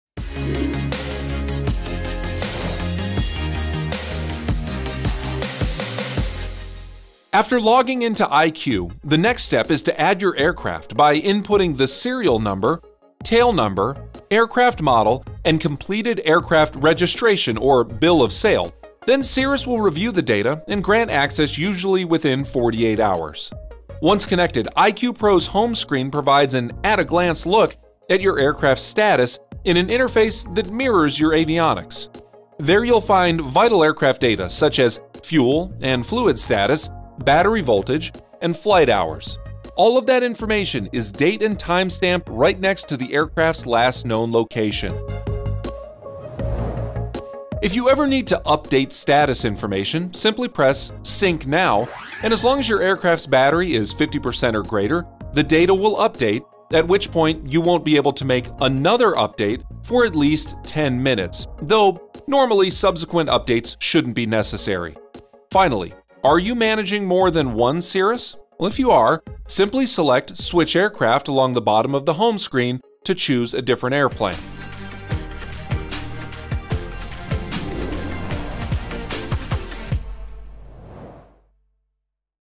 CIRRUS IQ PRO™ TRAINING: CONNECTING TO YOUR AIRCRAFT
Welcome to Cirrus IQ PRO. This video will go over how to connect your aircraft to your account, the aircraft data you will be able to view, how to update your status, and how to handle multiple aircraft in your account.

0:00 Connecting Your Aircraft
0:24 Cirrus IQ PRO Home screen
0:47 How to Update Status
1:06 Managing Multiple Aircraft